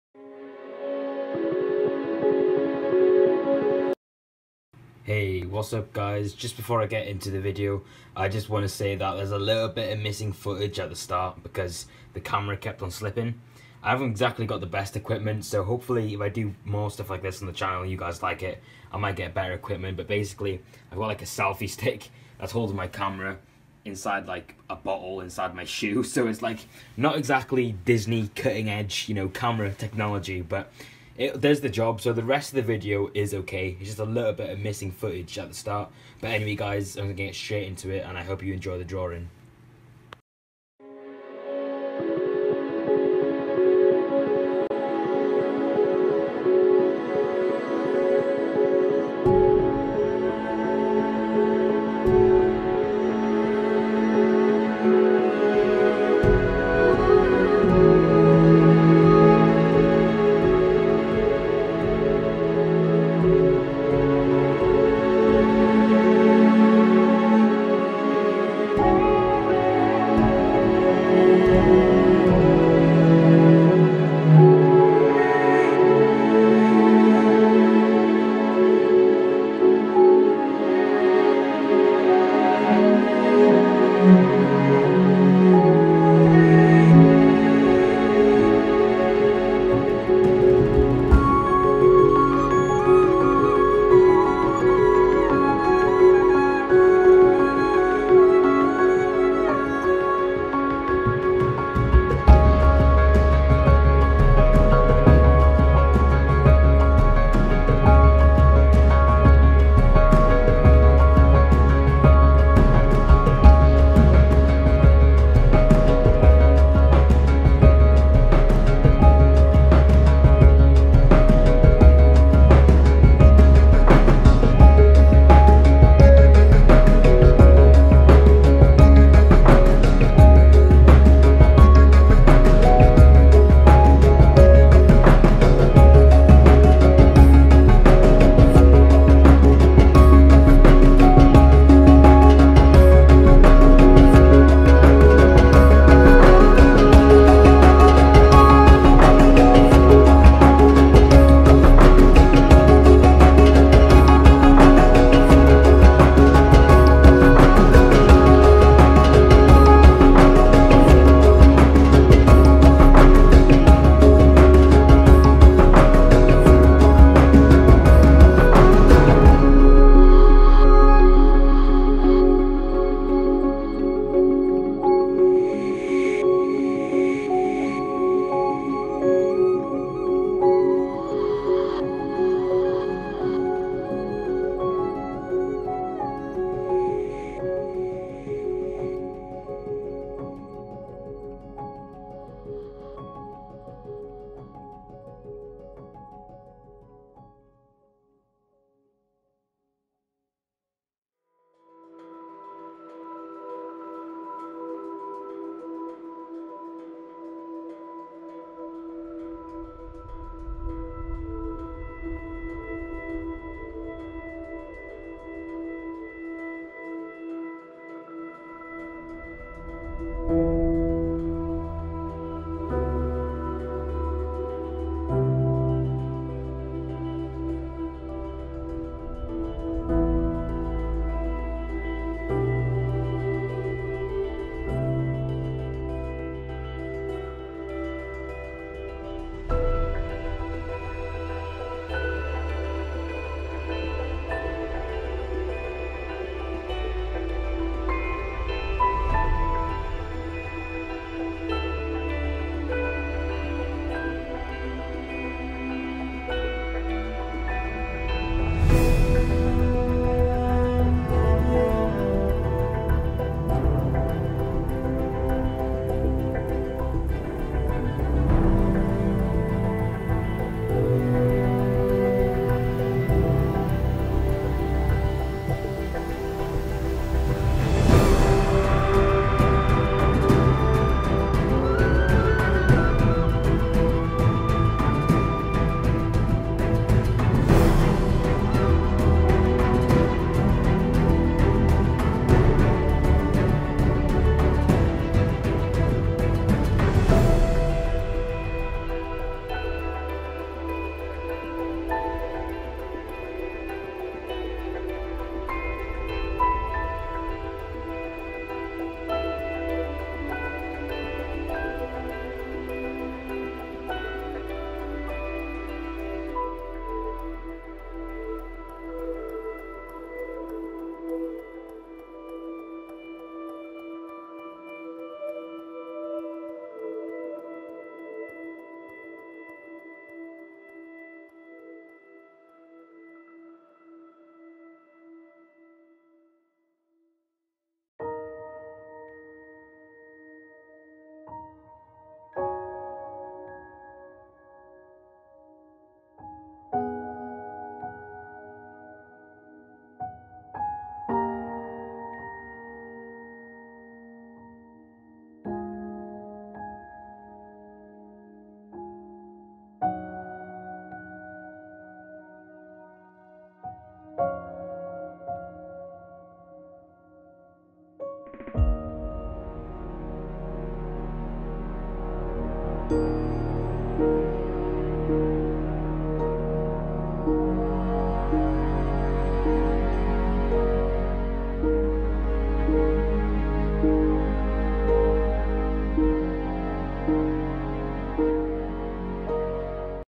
Lagertha Drawing Vikings Speed Drawing Timelapse (Katheryn Winnick)
DescriptionMy speed drawing portrait of Lagertha from Vikings played by Katheryn Winnick. (Using graphite pencil)
Hope you enjoy the video! Let me know if I should do more things like this on my channel.